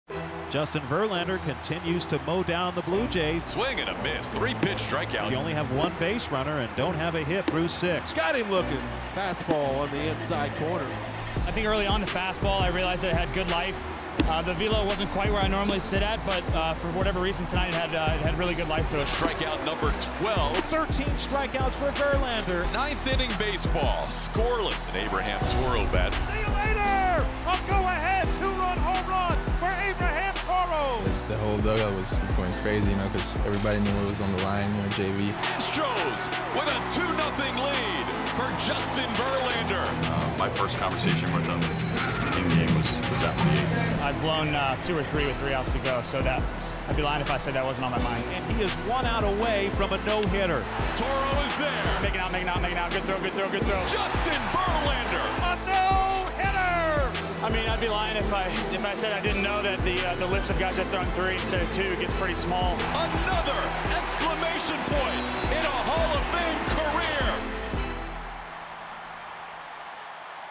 Greatest Moments of 2019: Verlander's 3rd no-hitter
Justin Verlander further cemented his Hall of Fame legacy on September 1st with his 3rd career no-hitter.